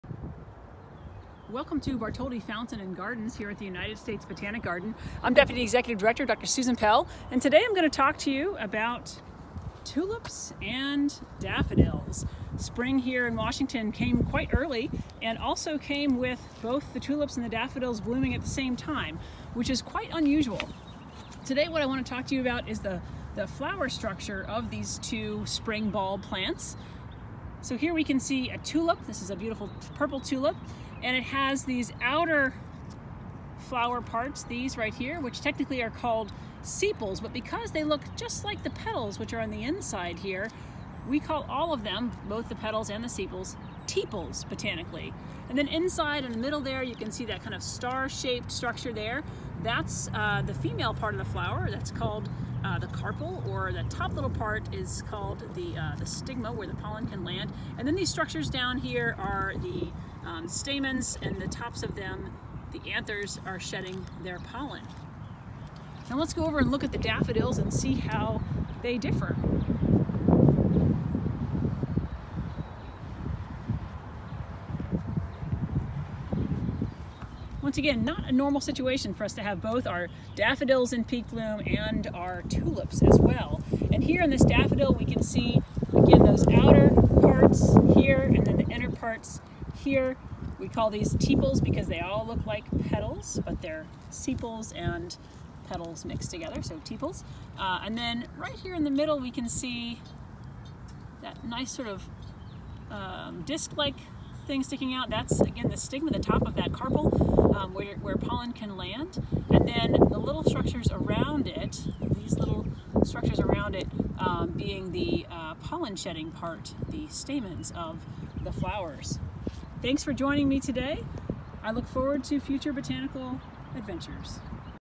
Tulip and Daffodil Morphology
Tepals, anyone? Join us for a quick look at the flower morphology of tulips (Tulipa sp.) and daffodils (Narcissus sp.)!